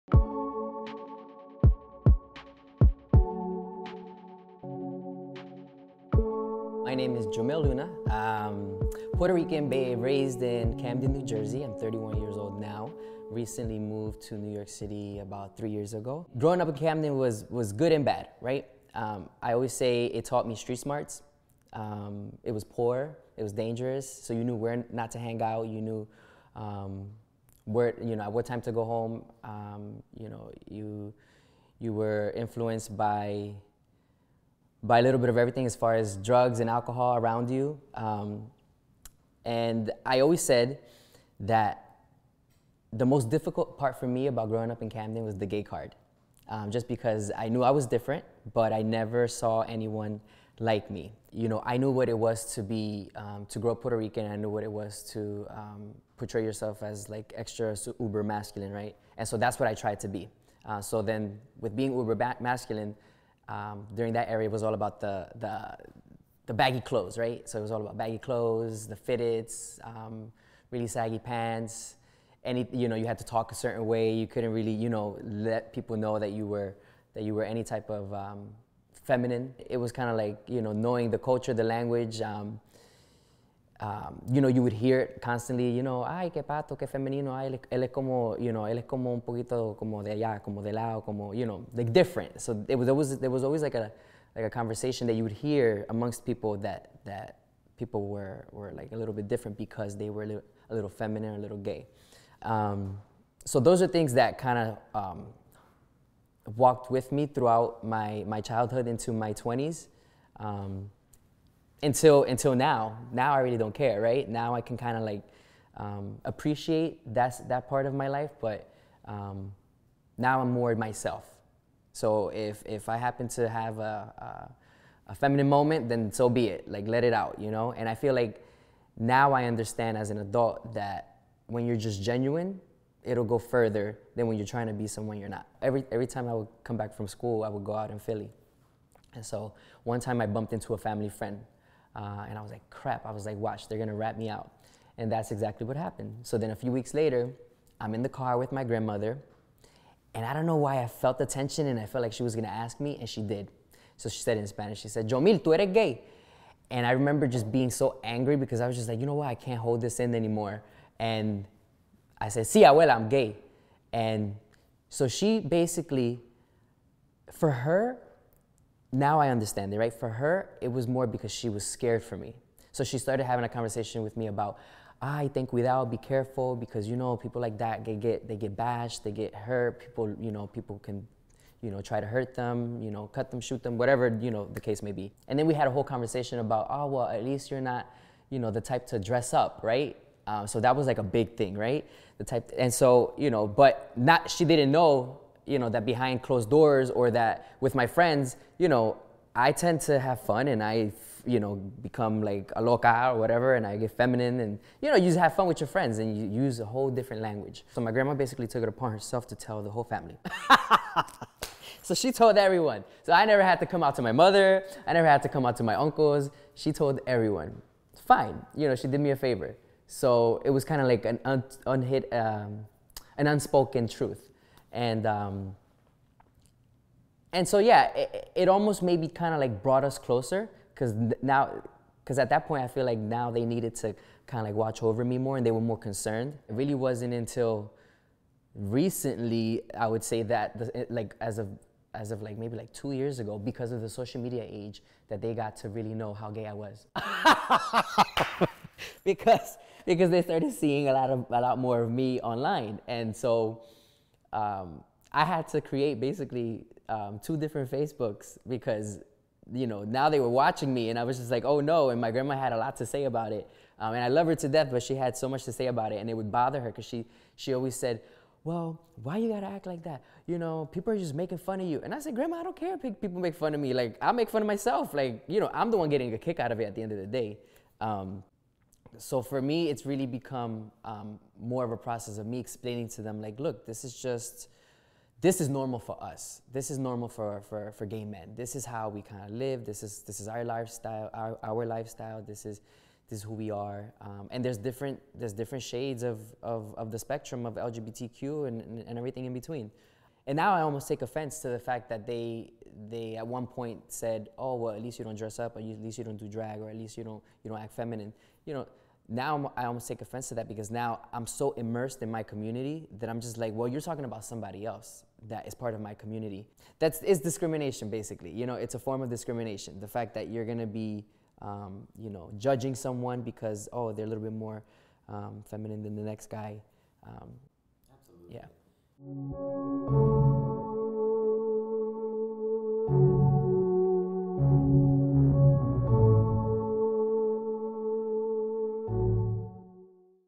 Other Boys NYC: Jomil Luna
Jomil Luna is a public health professional, advocate and community organizer. He was born in Camden, NJ into a Puerto Rican family. During this episode, Jomil speaks about how his environment shaped his understanding of self-expression. He reflects on masculinity within the Puerto Rican community and how he has learned to unapologetically be himself around his family and community.